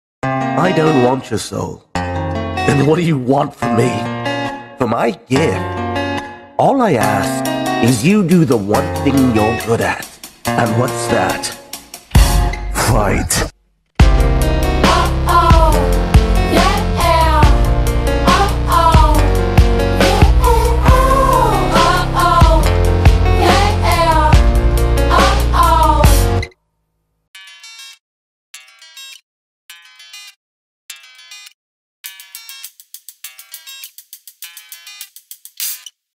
I Don’t Want Your Soul ✧ AASB Animation
I don't want your soul, I want you to do the one thing you're good at... fight.

Doom and Damien strike a bargain, and Doom joins the Guard. Her training begins c:

∘₊✧─── All videos are PG-13 ───✧₊∘
Art made entirely by me, Sara. Do not claim as your own, or use for any purpose ♥